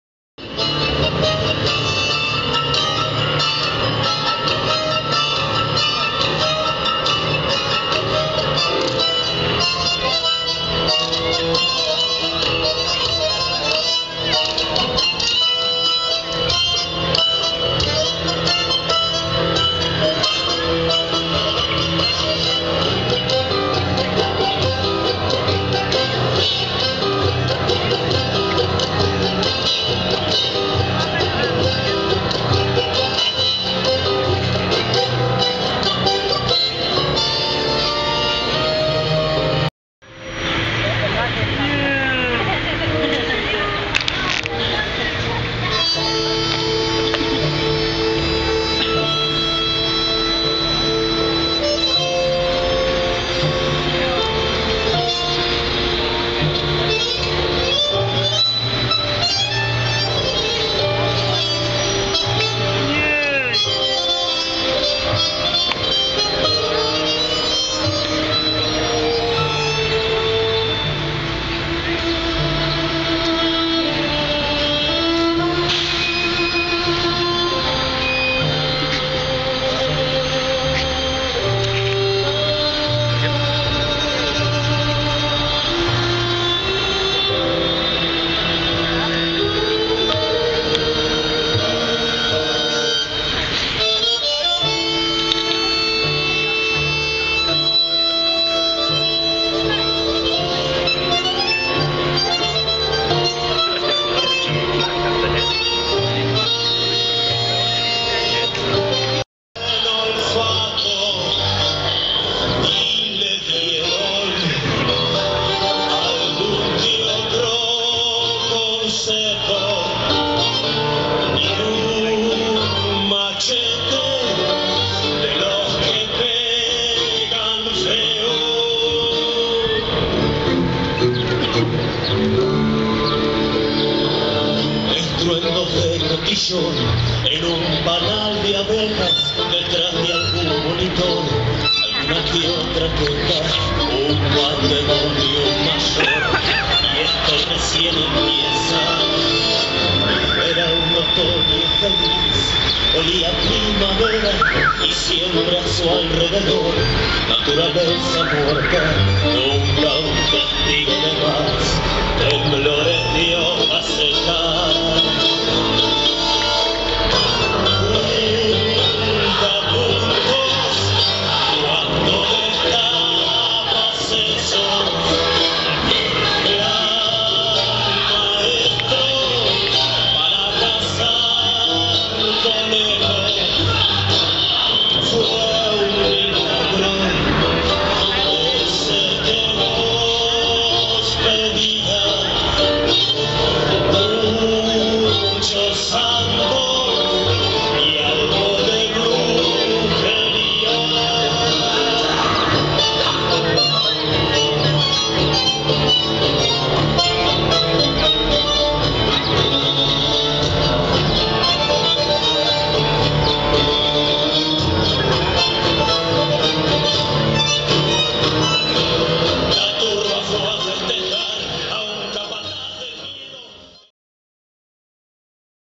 Tango In The Octagon
Peso Medio from Argentina perform in the Octagon to launch the Otago Festival of The Arts 2010.